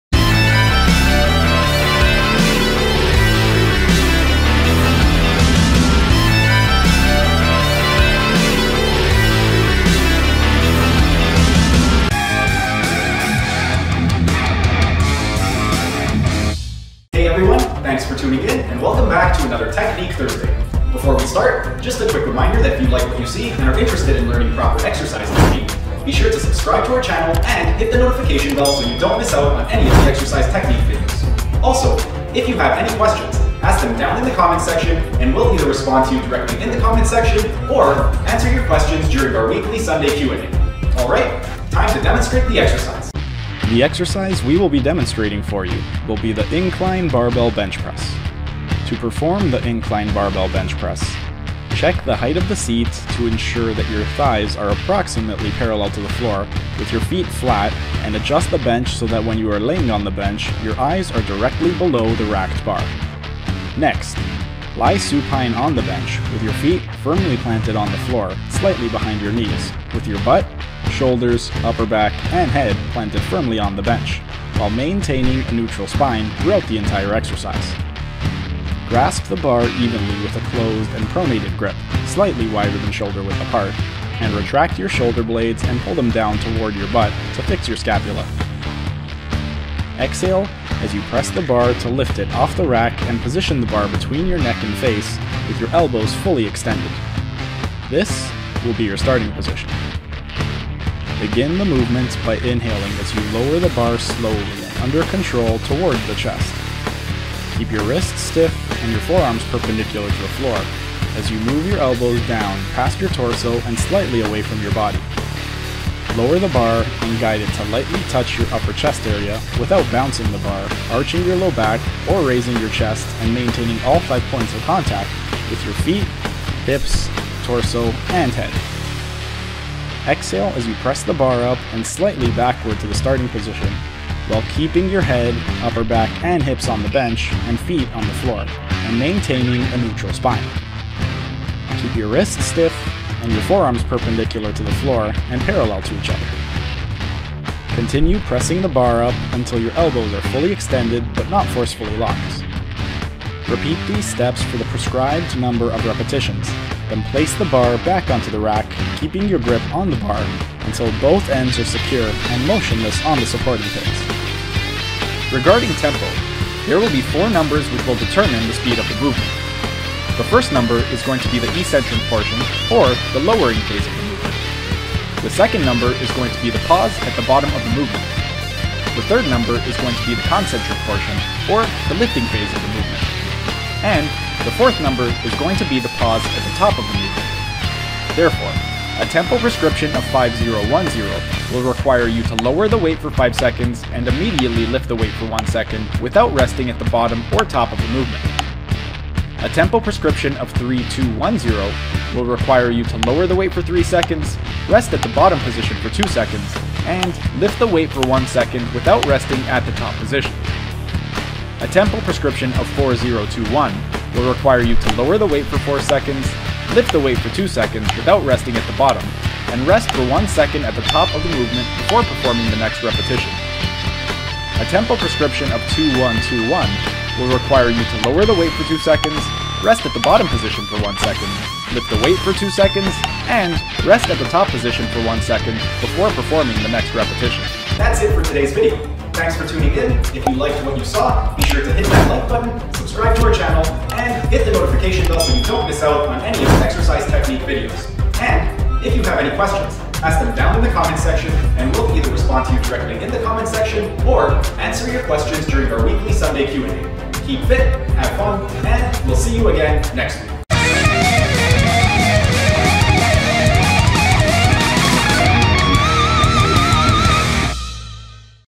BEST Upper Chest Exercises to BUILD MUSCLE: How to INCLINE BENCH PRESS
Do you want to know one of the best upper chest exercises to build muscle? The Incline Barbell Bench Press is one of the best chest exercises! Holistic Health Coach, Stephen Daniele, demonstrates how to perform Incline Barbell Bench Press to get toned arms and strong chest in this tenth installment of Technique Thursday! And if you'd like to learn more exercise techniques, subscribe to our channel and hit the notification bell so you don't miss out o any of the technique videos, which will be uploaded every Tuesday and Thursday at 2pm!!!

⏰TIMESTAMPS⏰
0:00 Quick Front and Side View Demonstrations
0:17 Introduction
0:43 Setup Position
1:41 The Lowering Phase
2:10 The Lifting Phase
2:45 Explanation of Tempo
3:08 Example of a 5-0-1-0 Tempo
3:19 Example of a 3-2-1-0 Tempo
3:31 Example of a 4-0-2-1 Tempo
3:44 Example of a 2-1-2-1 Tempo
3:57 Thanks for Tuning In. Please Like, Share, Comment, and Subscribe!!!

To perform the Incline Barbell Bench Press, check the height of the seat to ensure that your thighs are approximately parallel to the floor with your feet flat and adjust the bench so that when you are laying on the bench your eyes are directly below the racked bar. Next, lie supine on the bench with your feet firmly planted on the floor, slightly behind your knees, with your butt, shoulders, upper back, and head planted firmly on the bench, while maintaining a neutral spine throughout the entire exercise. Grasp the bar evenly with a closed and pronated grip, slightly wider than shoulder-width apart, and retract your shoulder blades and pull them down towards your butt to fix your scapula. Exhale as you press the bar to lift it off the rack and position the bar between your neck and face with your elbows fully extended. This will be your starting position.

Begin the movement by inhaling as you lower the bar slowly and under control toward the chest. Keep your wrists stiff and your forearms perpendicular to the floor, as you move your elbows down past your torso and slightly away from your body. Lower the bar and guide it to lightly touch your upper chest area, without bouncing the bar, arching your low back, or raising your chest and maintaining all five points of contact with your feet, hips, torso, and head. Exhale as you press the bar up and slightly backward to the starting position while keeping your head, upper back, and hips on the bench and feet on the floor and maintaining a neutral spine. Keep your wrists stiff and your forearms perpendicular to the floor and parallel to each other. Continue pressing the bar up until your elbows are fully extended, but not forcefully locked. Repeat these steps for the prescribed number of repetitions, then place the bar back onto the rack, keeping your grip on the bar until both ends are secure and motionless on the supporting pins.

Regarding tempo, there will be four numbers which will determine the speed of the movement. The first number is going to be the eccentric portion, or the lowering phase, of the movement. The second number is going to be the pause at the bottom of the movement. The third number is going to be the concentric portion, or the lifting phase, of the movement. And the fourth number is going to be the pause at the top of the movement.

Therefore, a tempo prescription of 5-0-1-0 will require you to lower the weight for 5 seconds and immediately lift the weight for 1 second, without resting at the bottom or top of the movement.

A tempo prescription of 3-2-1-0 will require you to lower the weight for 3 seconds, rest at the bottom position for 2 seconds, and lift the weight for 1 second without resting at the top position.

A tempo prescription of 4-0-2-1 will require you to lower the weight for 4 seconds, lift the weight for 2 seconds, without resting at the bottom, and rest for 1 second at the top of the movement before performing the next repetition.

A tempo prescription of 2-1-2-1 will require you to lower the weight for 2 seconds, rest at the bottom position for 1 second, lift the weight for 2 seconds, and rest at the top position for 1 second before performing the next repetition.

If you would like help perfecting your exercise technique, schedule a complimentary video consultation to see how we can help you with your form